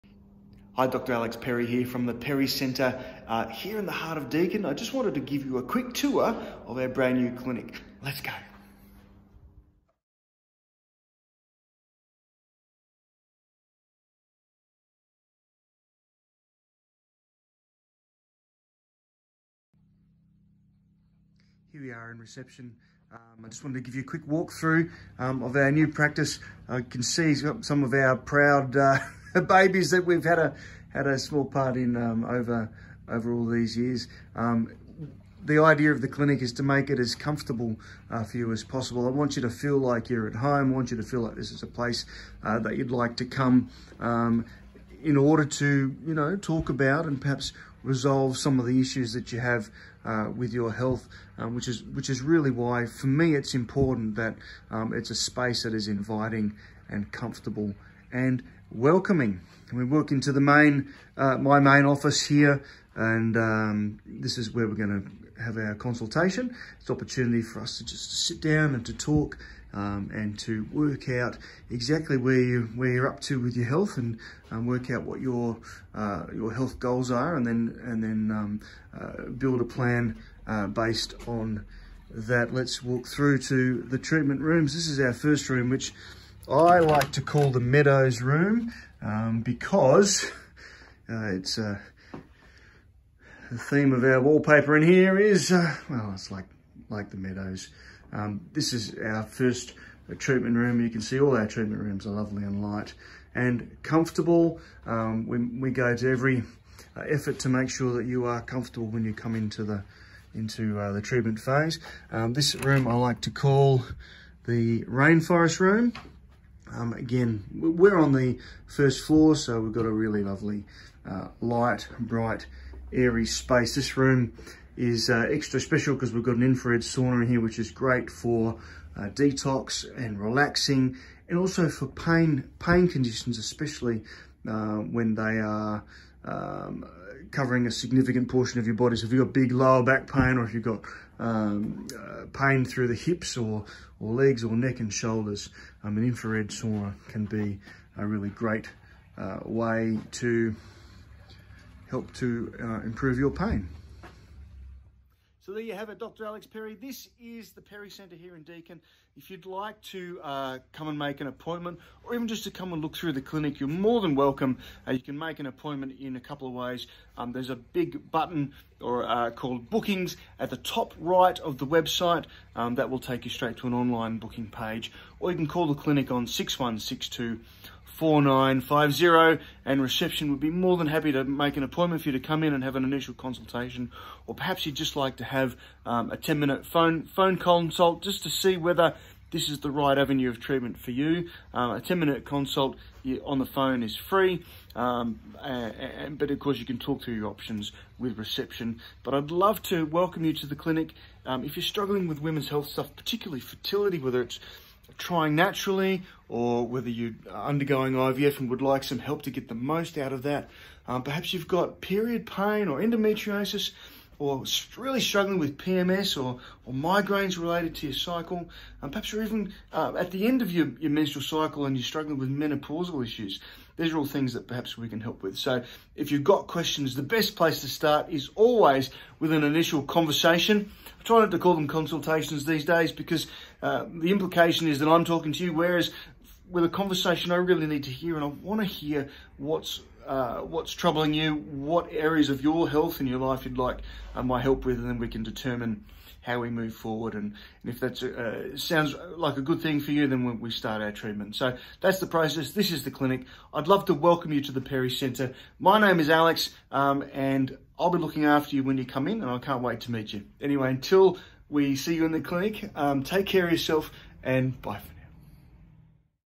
Clinic walk through
Walk with me through the new Perry Centre in Deakin. Lets go!